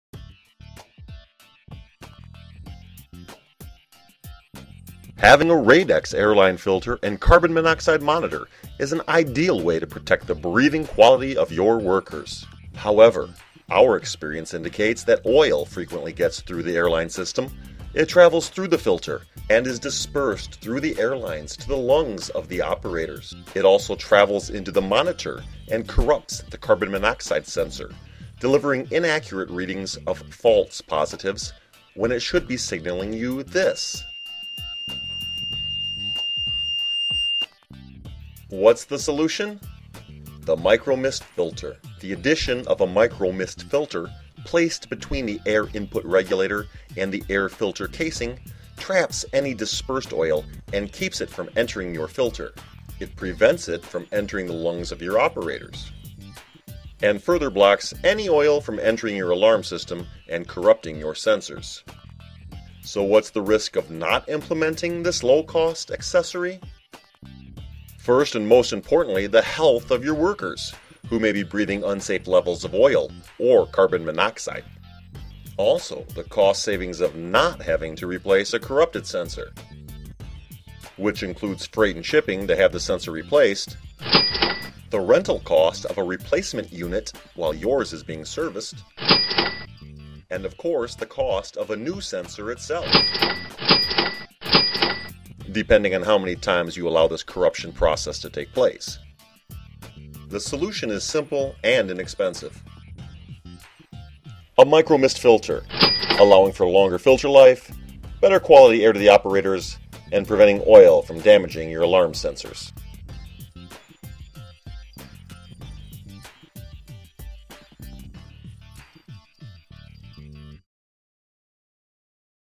The Genius of the Radex Micro Mist Filter
Place the Micro Mist Filter between the air input regulator and the air filter casing on your Radex Airline Filter and eliminate oil entering the system, operators airlines, or damaging the alarm sensor.